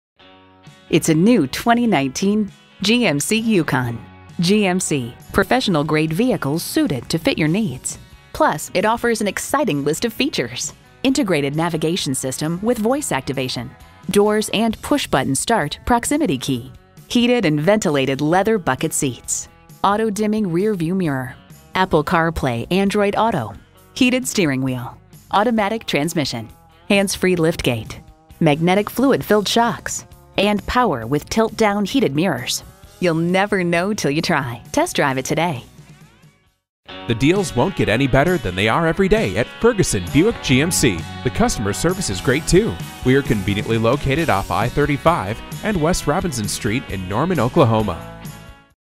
New 2019 GMC YUKON DENALI Norman OK Oklahoma-City, OK G1434 - SOLD
Year: 2019
Make: GMC
Model: YUKON DENALI
Engine: 6.2 Liter 8 Cylinder
Transmission: Automatic
Color: Crimson Red Tintcoat
Interior: Cocoa/Shale
Stock #: G1434
VIN: 1GKS2CKJ5KR128361

Address:
1015 North Interstate Drive